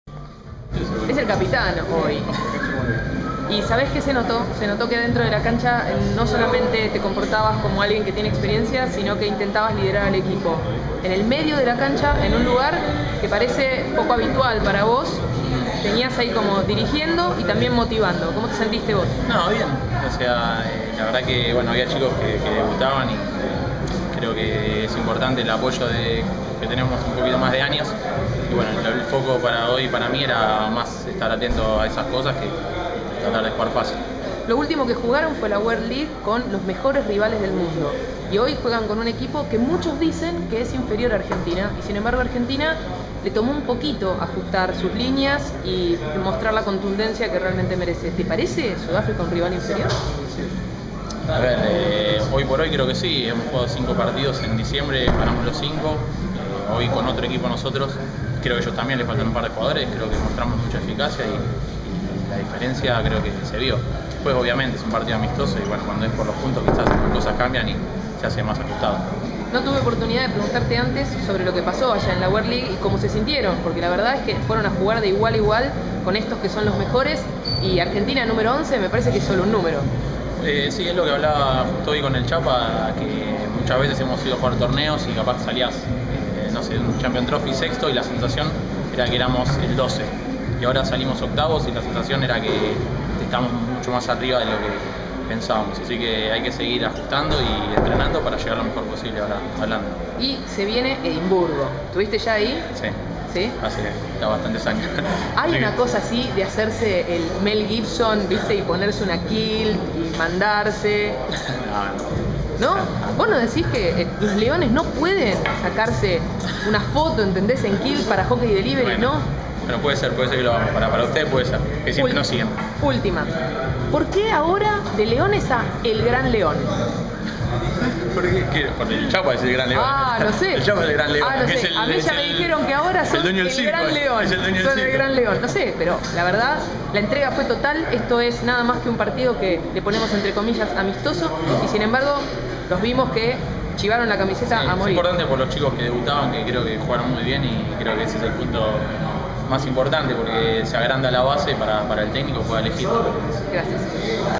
Test Match #1: ARG 6-0 RSA - Escuchá Matias Paredes EXCLUSIVO desde Mardel
Los Leones vencieron a su par de Sudafrica en el primero de la serie de Test Matches que se juegan en Mar del Plata como preparacion para el Mundial de La Haya 2014. Tras el encuentro, Matías Paredes, capitán en esta ocasión habló en EXCLUSIVA para Hockey Delivery. Escuchalo...